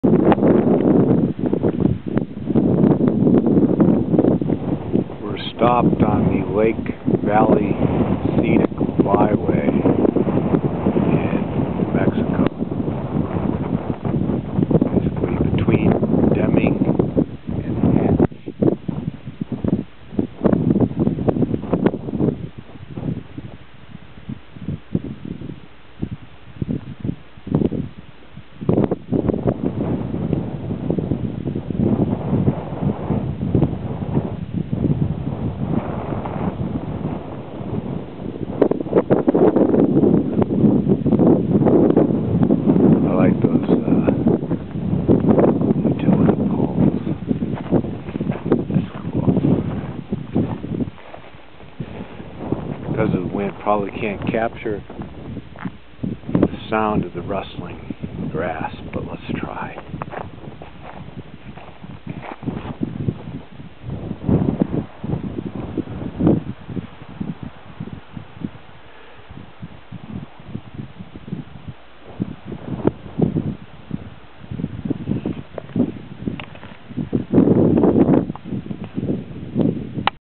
SPOT: Lake Valley Back Country Byway in New Mexico
This SPOT (Some Place Out There) in on NM Highway-27 in New Mexico.